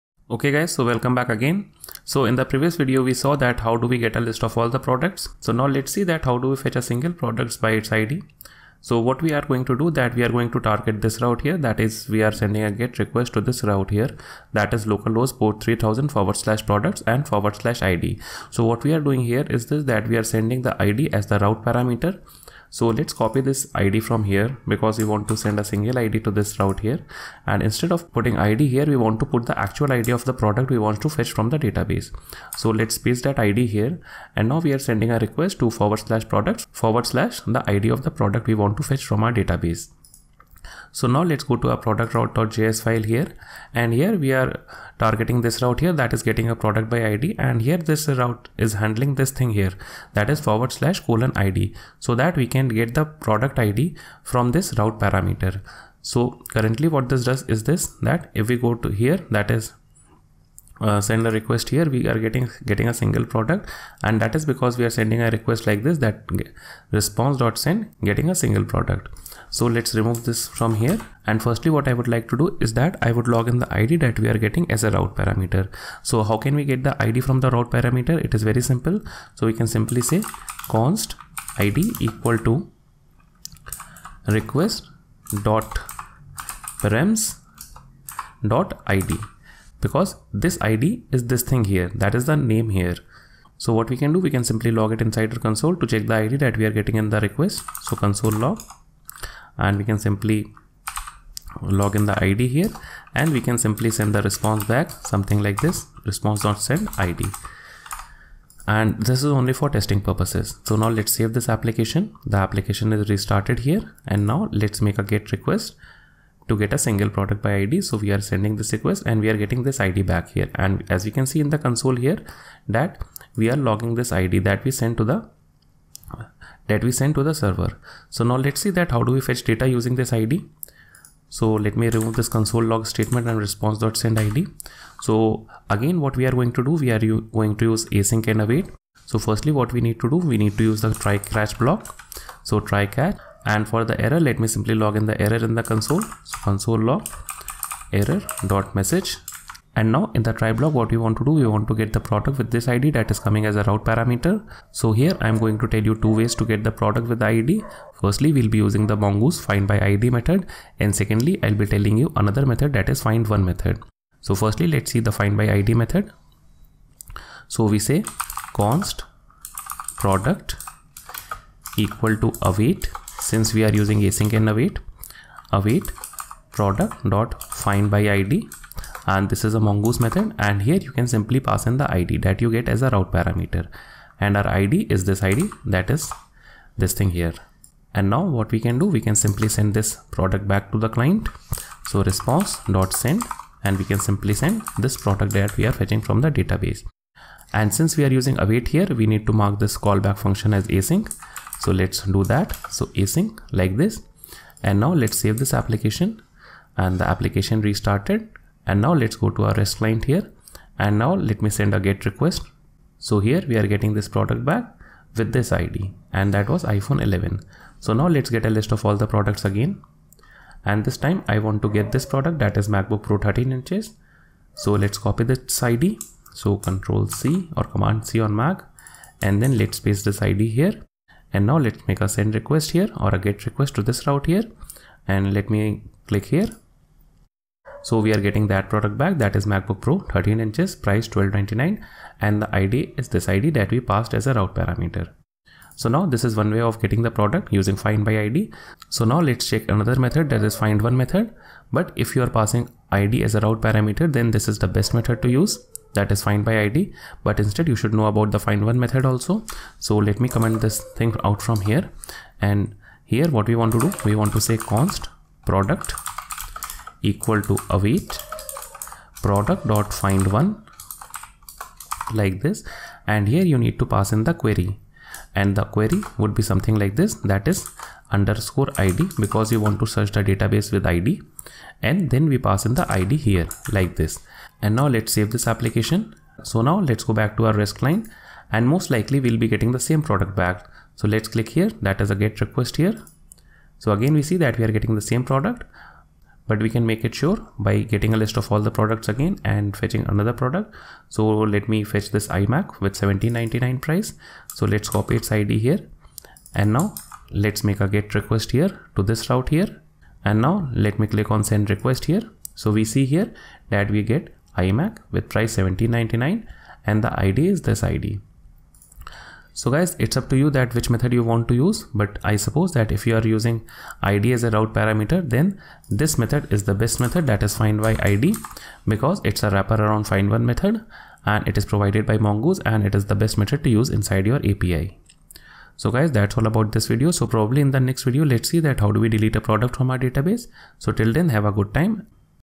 Getting a single Product | RESTful API using NodeJS and MongoDB
In this series you will learn how to build a RESTAPI using NodeJS and MongoDB.
You will learn the mongoose findById method to get a document from the database, we would also be talking about the findOne method.